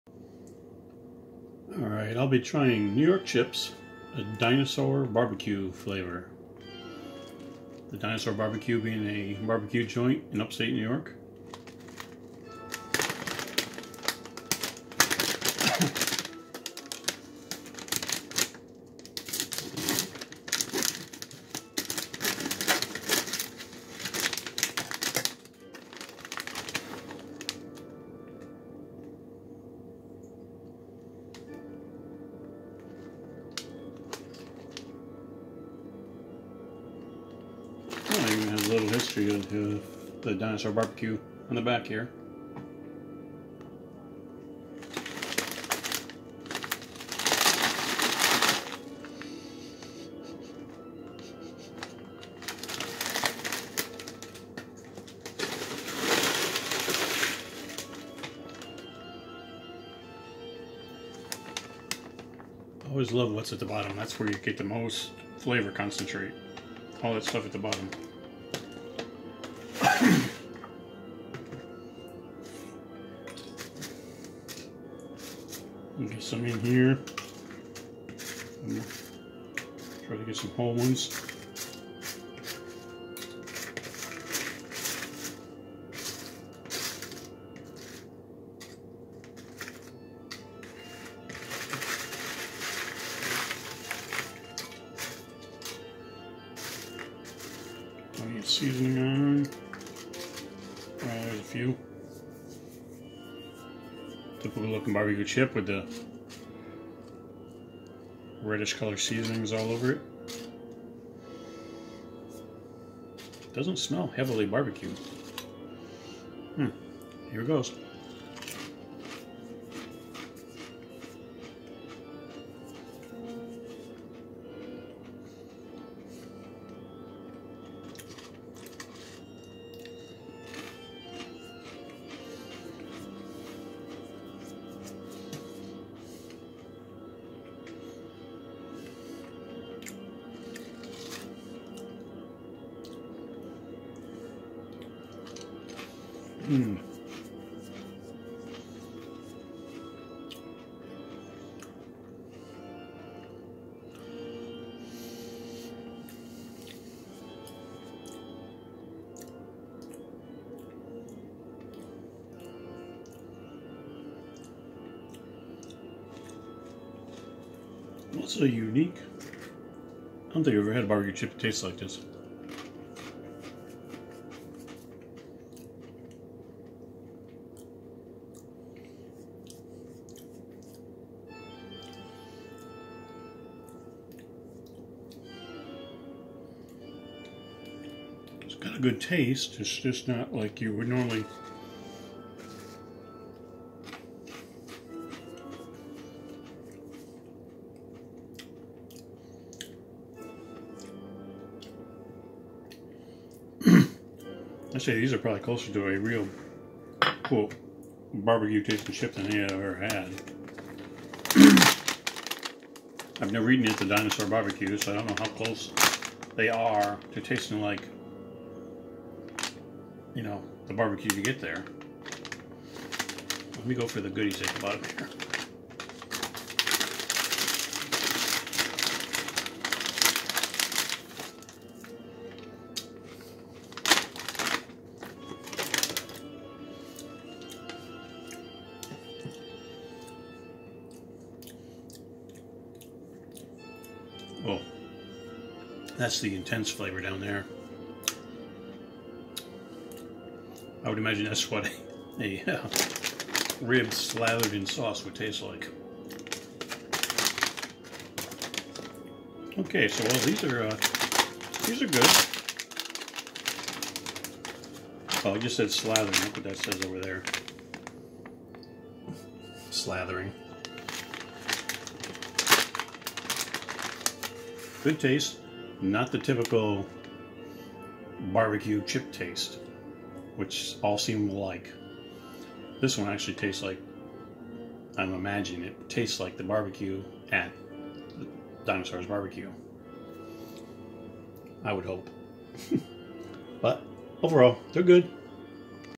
New York Chips Dinosaur Bar-B-Que Potato Chips Tasting
My Community poll ended in a three-way tie. So I chose the one with the nearest Best Before date.